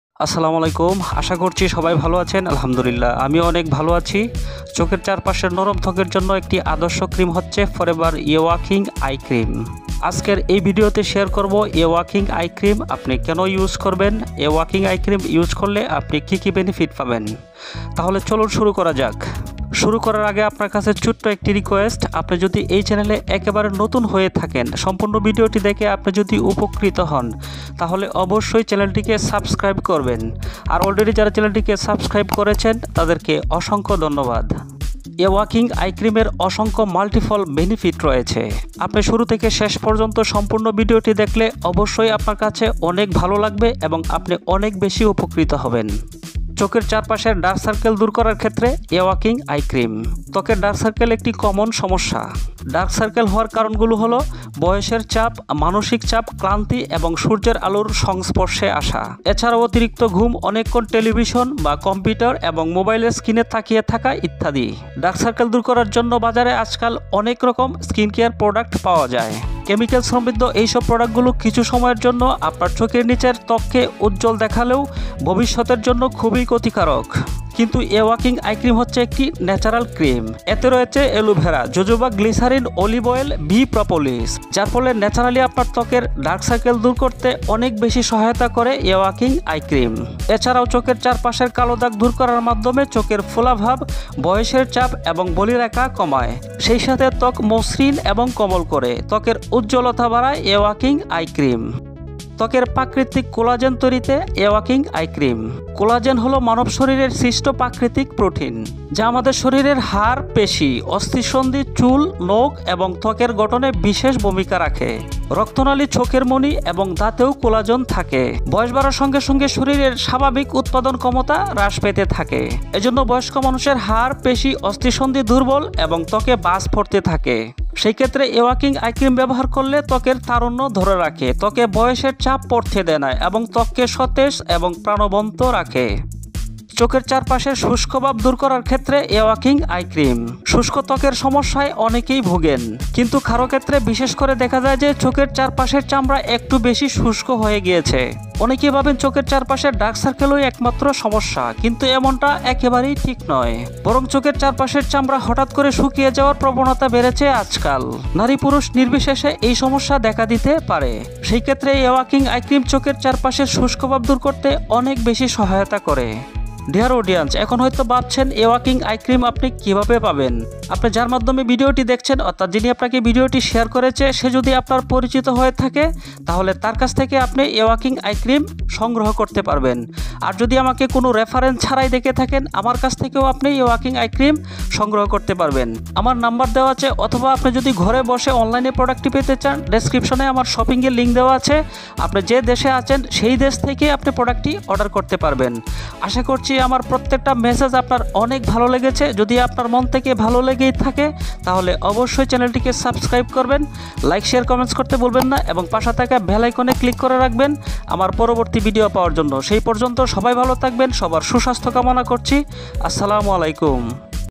Awakening Eye Cream Forever Living Products | Kamal Uddin
Forever’s Awakening Eye Cream combines powerful ingredients with collagen, aloe vera and peptide technology to improve and smooth the sensitive eye area, visibly reducing the appearance of wrinkles and dark circles.

Beautify the eye.
• Diminishes the appearance of puffiness and dark circles
• Significantly reduces appearance of wrinkles
• Eyes appear more rested, alert and rejuvenated
• Vegetarian friendly
• Gluten free

Awakening Eye Cream combines patented ingredients with peptide technology to improve and smooth the sensitive eye area, visibly reducing the appearance of lines, wrinkles and dark circles. This groundbreaking formula helps with normal collagen formation to promote optimal skin hydration and firmness. By supporting healthy collagen, awakening eye cream improves skin smoothness, resulting in plumper, firmer skin around the eye area. Awakening Eye Cream also contains a special peptide that has been shown to reduce the appearance of eye puffiness and dark circles in as little as fifteen days!
All these powerhouse ingredients make the most advanced eye cream to allow your beauty to shine. Have striking eyes that look brighter and younger with Awakening Eye Cream.

USAGE
Using your finger, gently dab a moderate amount below and around the eye area.

Md Kamal Uddin
Recognized Manager
Forever Living Products